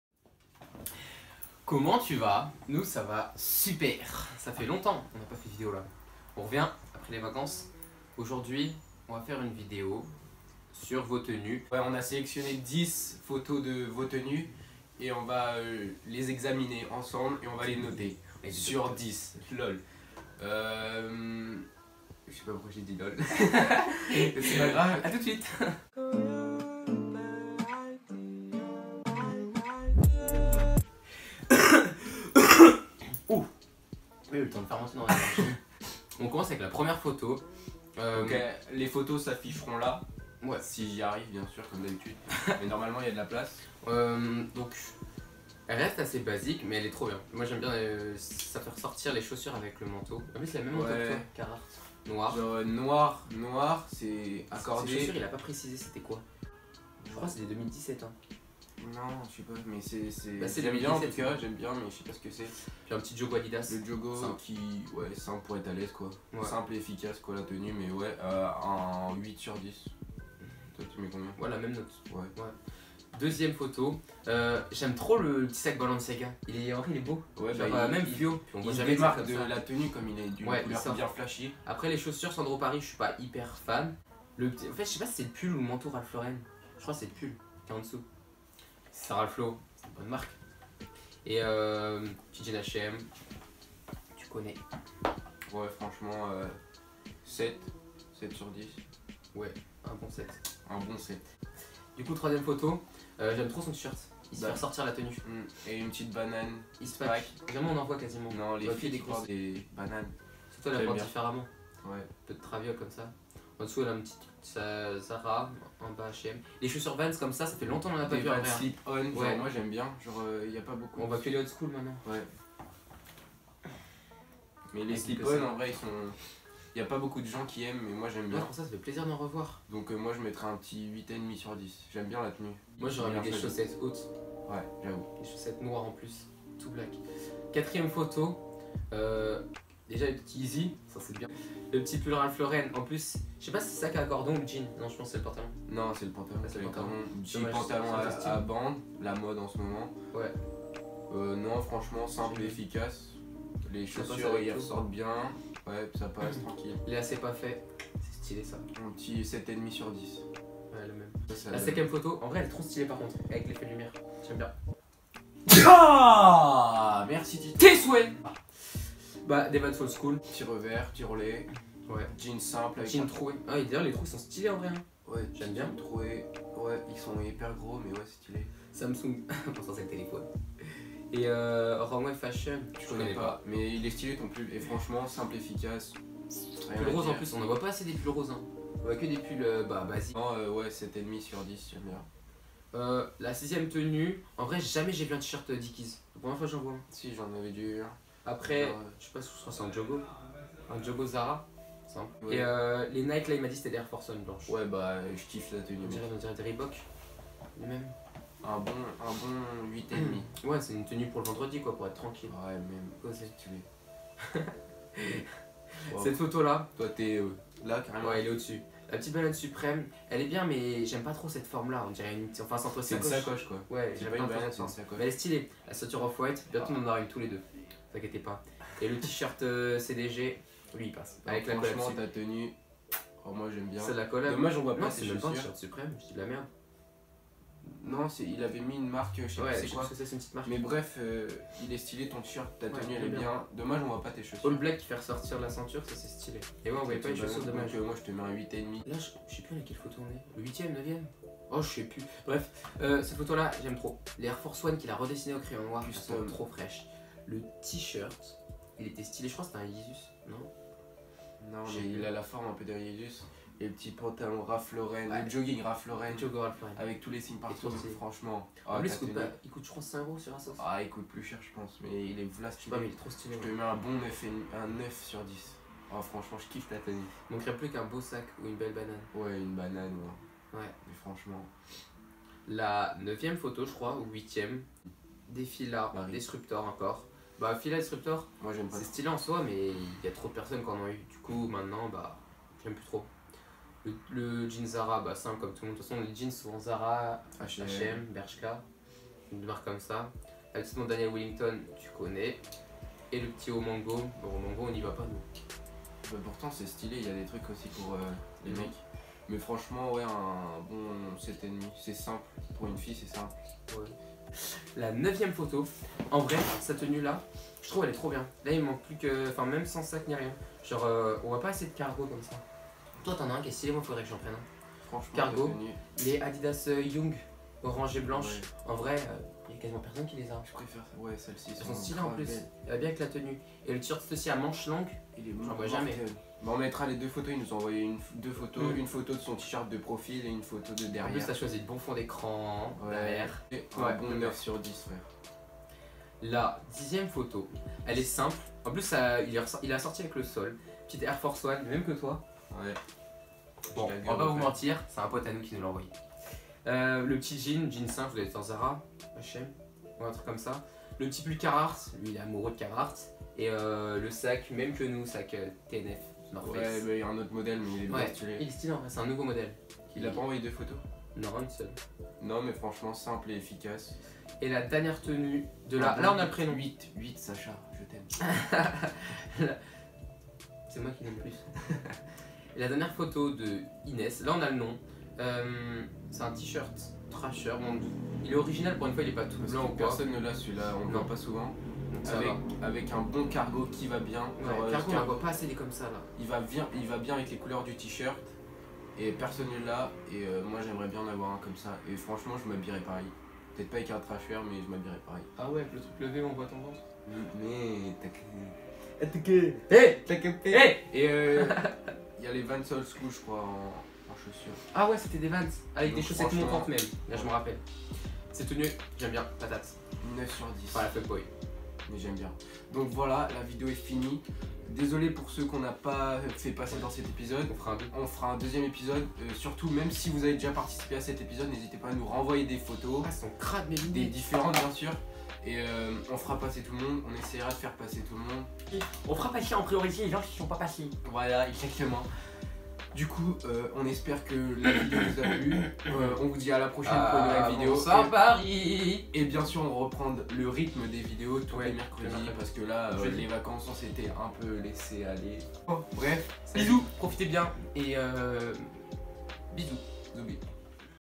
ON RÉAGIT A VOS TENUES ! 🧢👖👕👟
Coucou c'est La Kagane,

On a décidé aujourd'hui de vous présenter une vidéo dans laquelle on réagit a vos tenues !
Désole pour ce qui nous ont envoyé leur tenue mais ne sont pas passé dans cette vidéo, ne vous inquiétez pas on fera un deuxième épisode, pour ce qui on déjà participé n'hésitez pas a nous renvoyez d'autre tenue, on fera passé en priorité les gens qui ne sont pas passé ;))
N'hésitez pas a nous dire en commentaire ce que vous pensez des tenues, on répondra avec plaisir !

Vinted :lakaganeyt